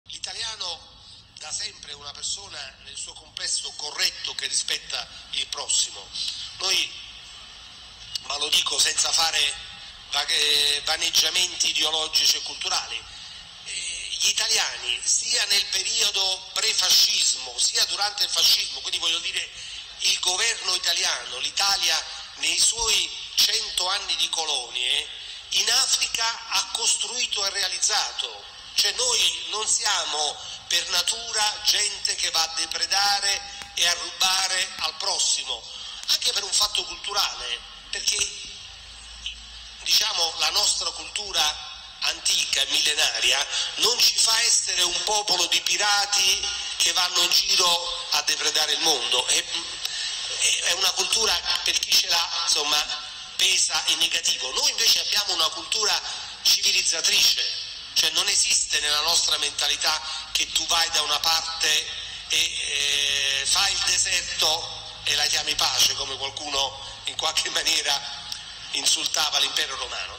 “Italiani non depredarono l’Africa”: viceministro Cirielli "cancella" i crimini del colonialismo
Il viceministro degli Esteri Edmondo Cirielli, di Fratelli d’Italia, partecipando a Fenix (l'evento organizzato da Gioventù nazionale) ha detto che l’Italia “nei suoi cento anni di colonie ha costruito e realizzato” perché “noi non siamo per natura gente che va a depredare e rubare”, anzi, quella italiana è “una cultura civilizzatrice”.